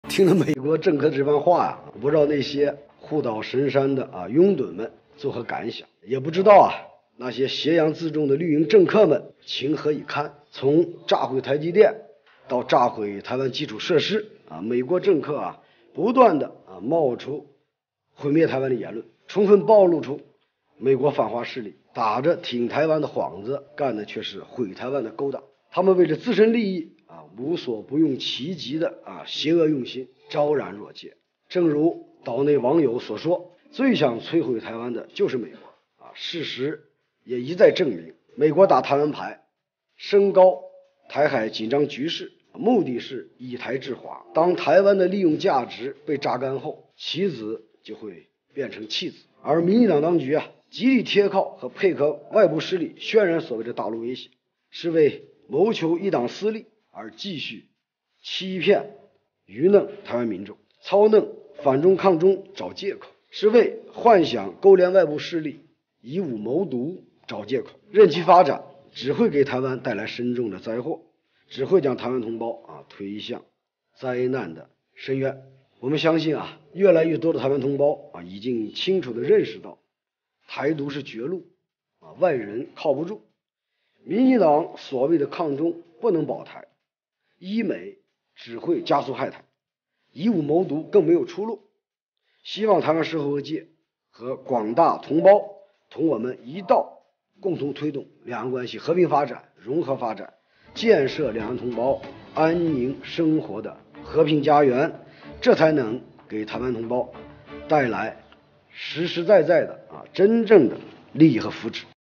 美议员扬言若台海发生冲突要炸毁台积电，国台办回应
美议员扬言若台海发生冲突要炸毁台积电 ，国台办回应#US lawmakers threaten to blow up TSMC if there is a conflict in the Taiwan Strait, Taiwan Affairs Office responds